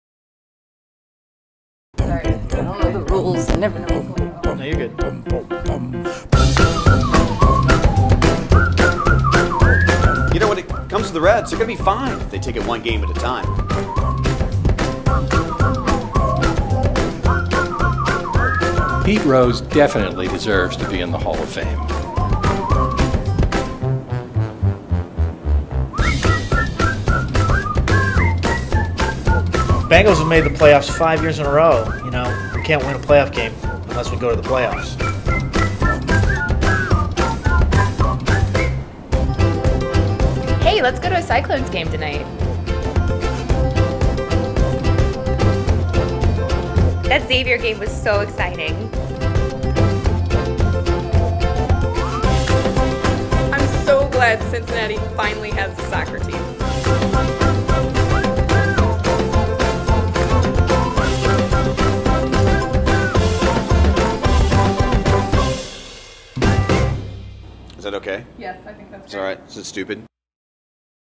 What Cincinnati sports fans are really thinking
Cincinnati sports fans stick up for their teams even at the worst of times. But that might not describe how they really feel...